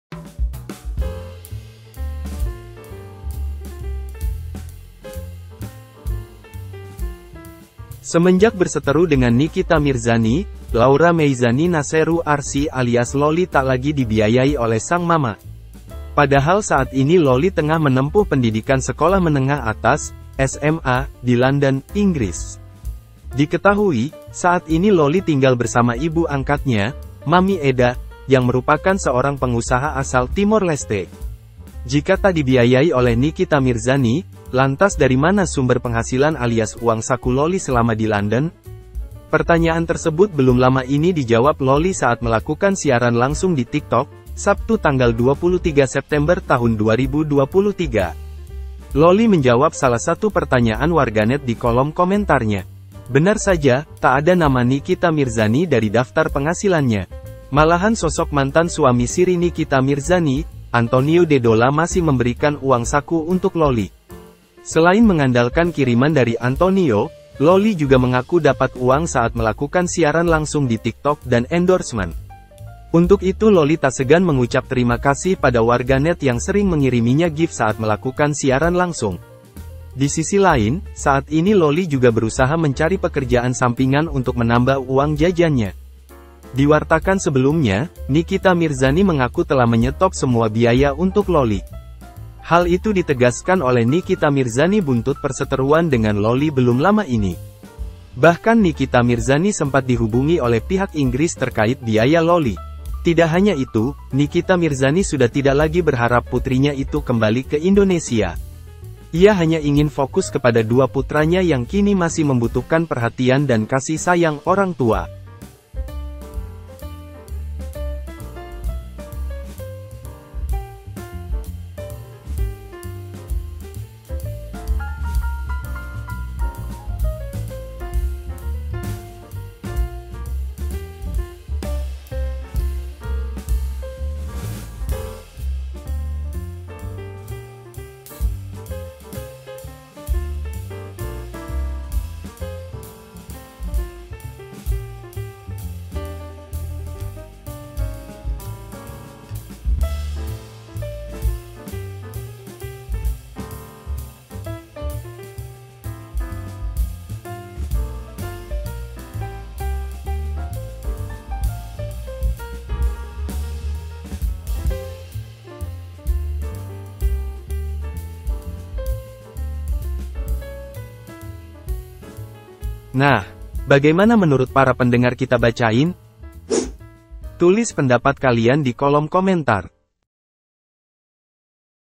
Lolly Ungkap Sumber Penghasilan Sejak Tak Dibiayai Nikita Mirzani, Ternyata Masih Ada Antonio Dedola
kumpulan berita artis

HAI PARAPENDENGAR!